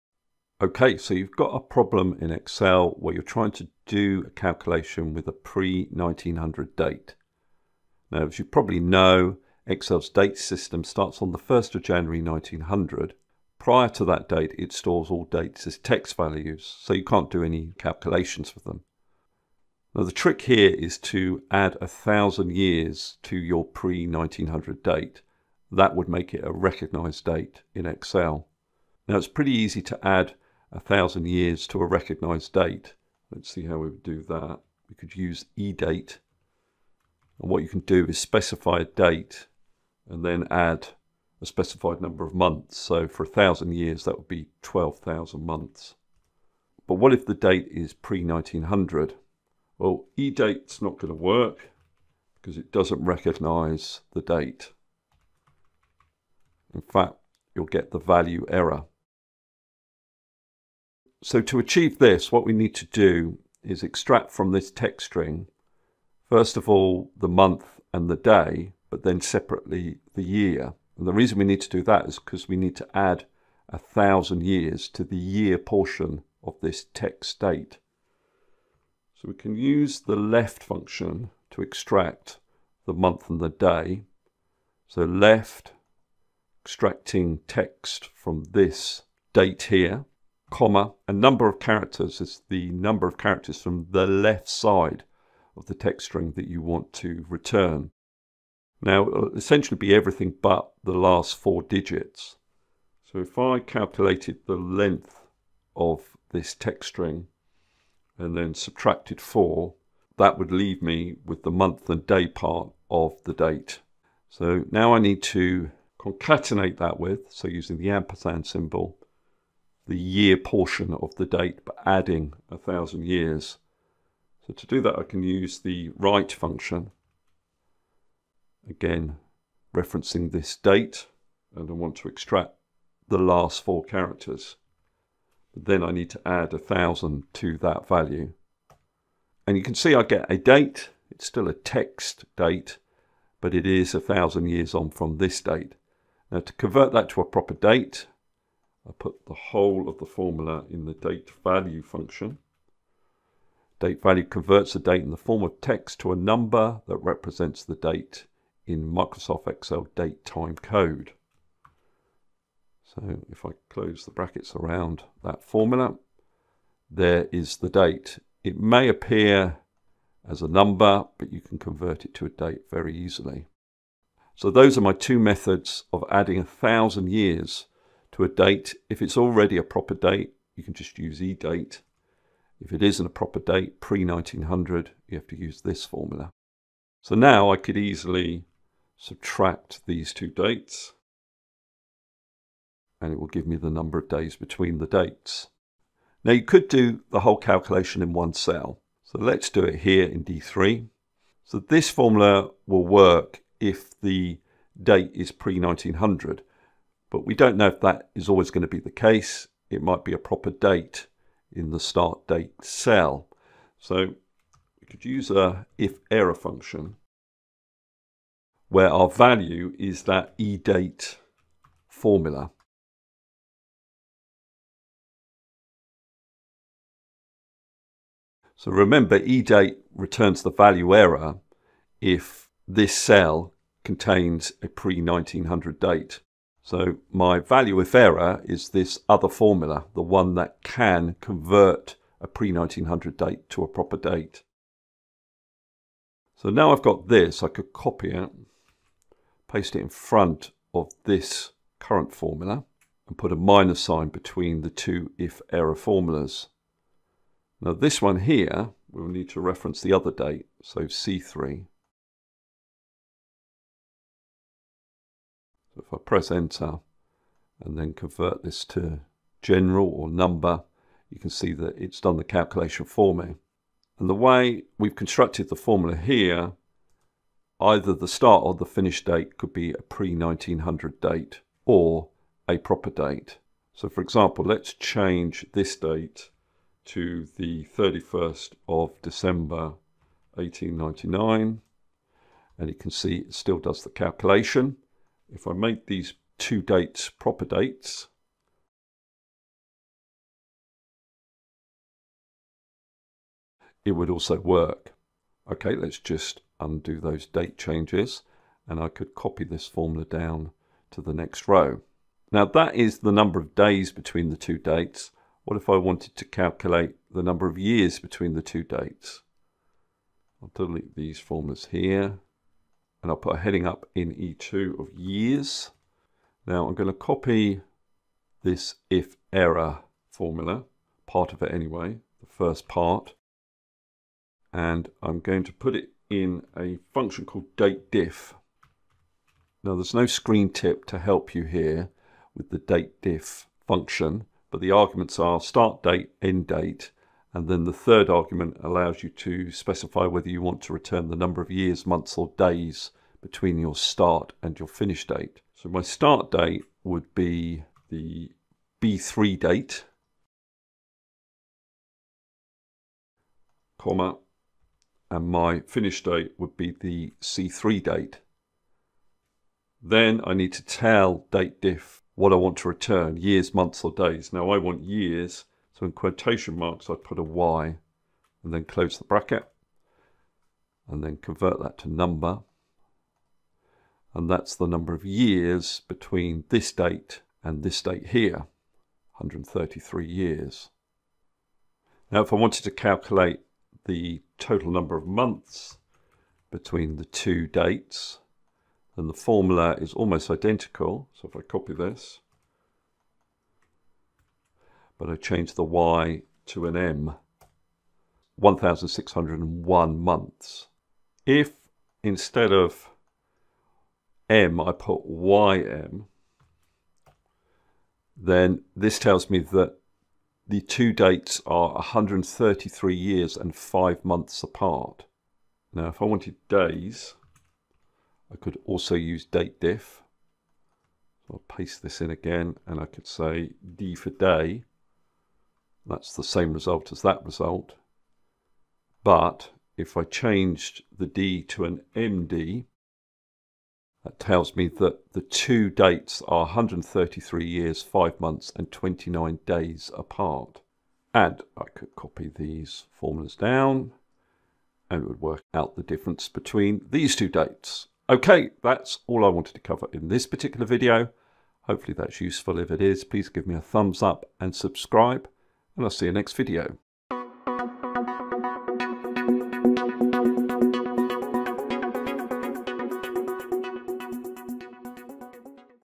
Can Excel Handle Dates Before 1900 (Yes!) | Difference in Years, Month or Days
In this video I demonstrate how to perform calculations on dates before 1900.  As you may know Excel does not recognise dates before the 01/01/1900.  However if you add 1000 years to a pre 1900 date you will get a date that Excel does recognise.

Table of Contents:
00:00 Introduction
00:28 Add a 1000 years to a recognised date using EDATE
01:01 Add a 1000 years to a pre 1900 date
03:06 Calculate difference in days between two dates
05:22 Calculate difference in years between two dates
06:31 Calculate difference in months between two dates